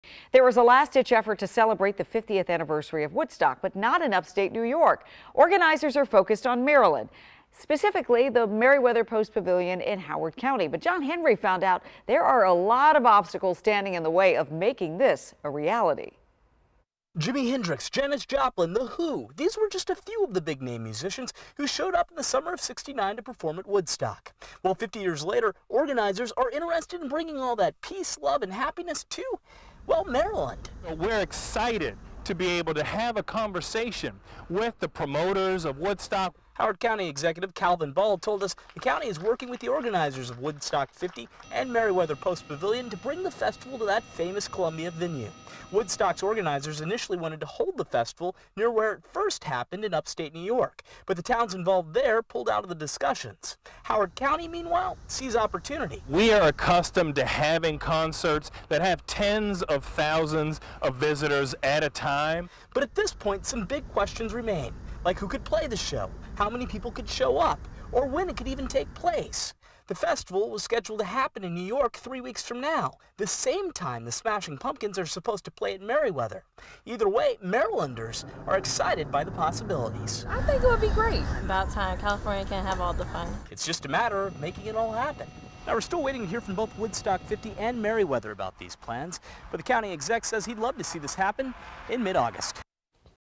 Howard County officials work to bring 'Woodstock 50' to Maryland
Howard County Executive Calvin Ball says county leaders are working with festival organizers and the Merriweather Post Pavilion to bring Woodstock to Columbia. Bloomberg reports it will take place Aug. 16 through 18 at the Merriweather Post Pavilion, an outdoor amphitheater there.